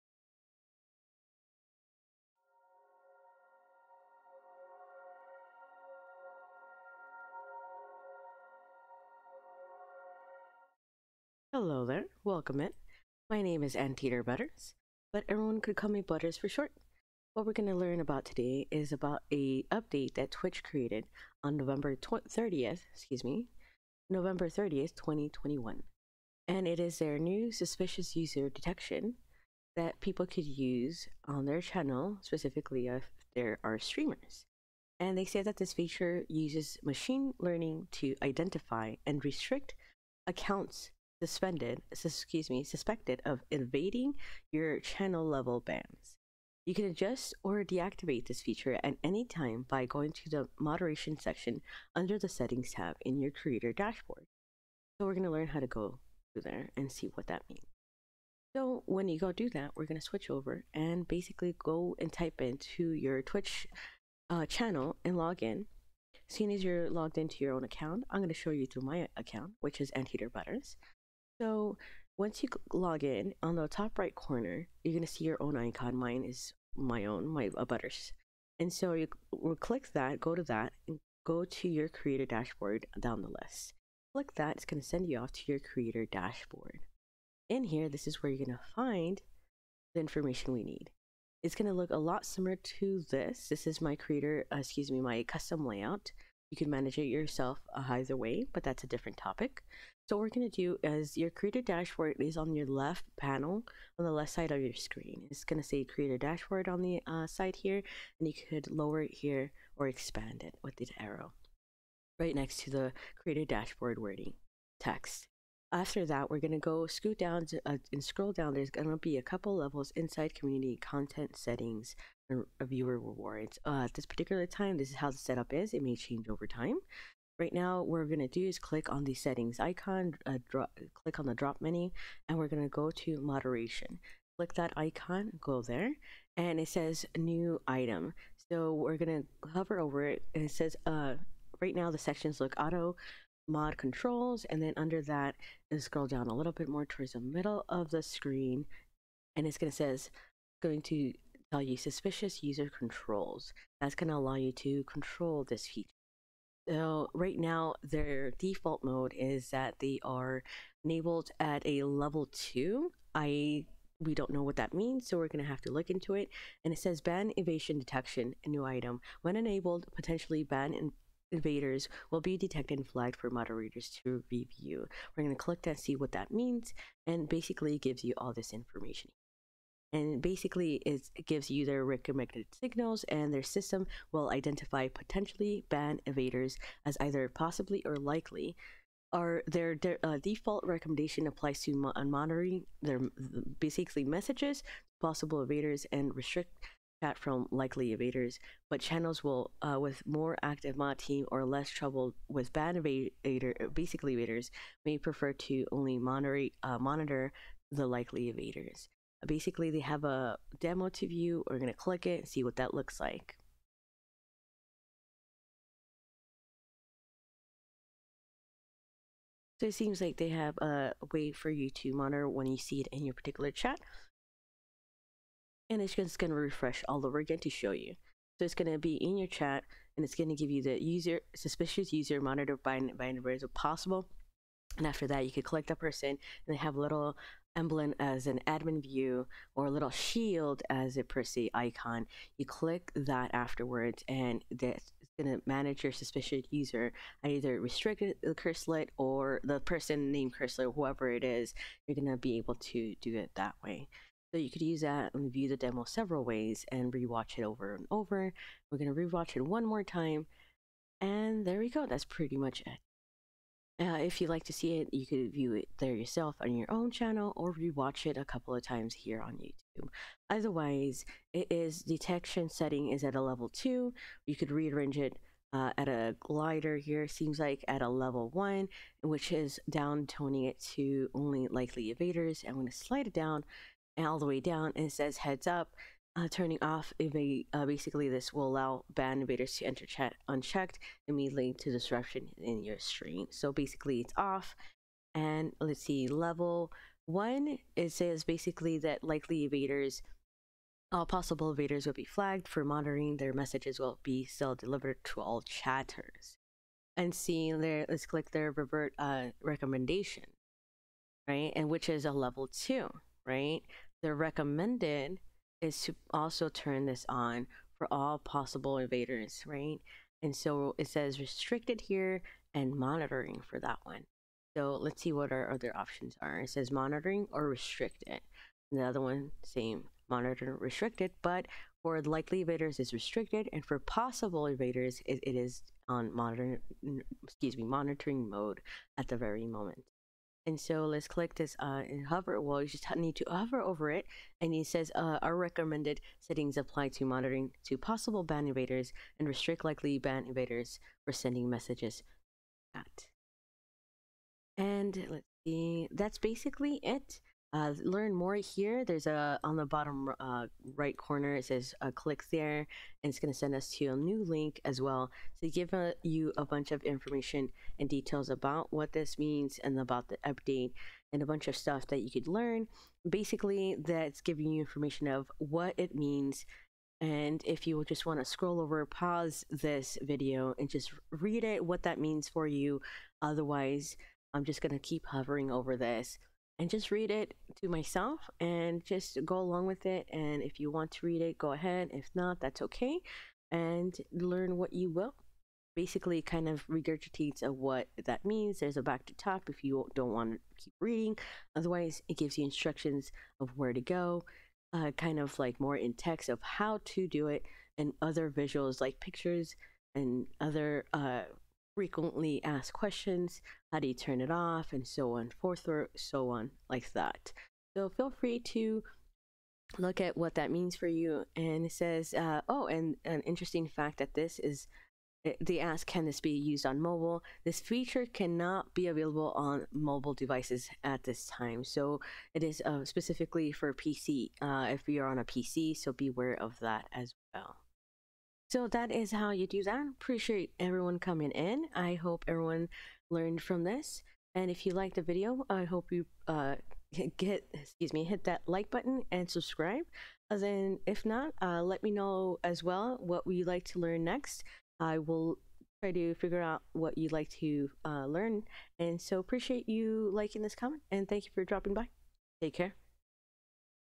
How to Enable and Disable Suspicious User Detection on Twitch
Twitch made an update on November 30, 2021.

Under Twitch Creator Updates, "Keep Ban Evaders Out of Your Channel" mentions that "Suspicious User Detection is now enabled for your channel. This feature uses machine learning to identify and restrict accounts suspected of evading your channel-level bans. You can adjust or deactivate this feature at any time by going to the “Moderation” section under the “Settings” tab in your Creator Dashboard."

To learn more, Twitch has a to learn more section, with headline "Battling Ban Evasion with Machine Learning" and a introduces their new tool new tool on Twitch for Content Creators, "Suspicious User Detection, a tool we built to help Creators and Mods catch and take action against users who are trying to evade channel-level bans.

When you ban someone from your channel, they should be banned from your community for good. Unfortunately, bad actors often choose to create new accounts, jump back into Chat, and continue their abusive behavior. Suspicious User Detection, powered by machine learning, is here to help you identify those users based on a number of account signals. By detecting and analyzing these signals, this tool will flag suspicious accounts as either “likely” or “possible” channel-ban evaders, so you can take action as needed. Here’s what happens in each case:

For “likely” ban evaders, messages won’t be sent to Chat at all. That said, they’ll be visible to Creators and Mods so they can choose to leave the restriction as it is, monitor the user, or ban them from the channel.

For “possible” ban evaders, messages will appear in Chat normally, but the account will be flagged to the Creator and their Mods so they can monitor the user and restrict them from chatting if needed.

You can also adjust how messages from each group are treated — for example if you want to be extra cautious, you can restrict messages from both likely and possible ban evaders from appearing in Chat.

We’re turning Suspicious User Detection on by default for all channels so that streamers can focus more on their content and their communities. You can, of course, adjust or deactivate this tool from the channel’s moderation settings page. You can also manually add users that you’d like to monitor more closely, either from the user’s Viewer Card or the new Suspicious Users widget in Mod View.

One thing to prepare for, particularly around launch, is that no machine learning will ever be 100% accurate, which means there is a possibility of false positives and false negatives. That's why Suspicious User Detection doesn’t automatically ban all possible or likely evaders. You’re the expert when it comes to your community, and you should make the final call on who can participate. The tool will learn from the actions you take and the accuracy of its predictions should improve over time as a result."  (Information used from Twitch Safety News).